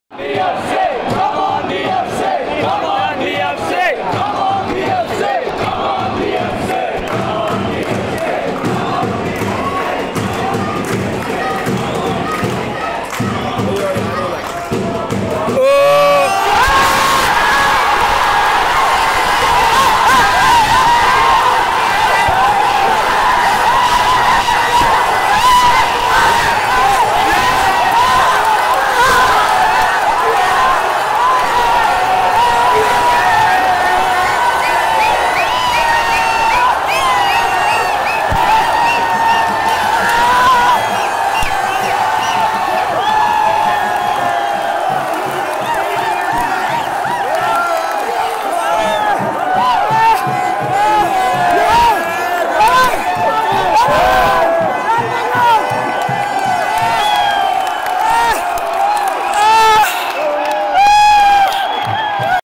Rahul bheke's header | Bengaluru FC vs FC Goa | ISL final | Mumbai football arena
Bheke's winning header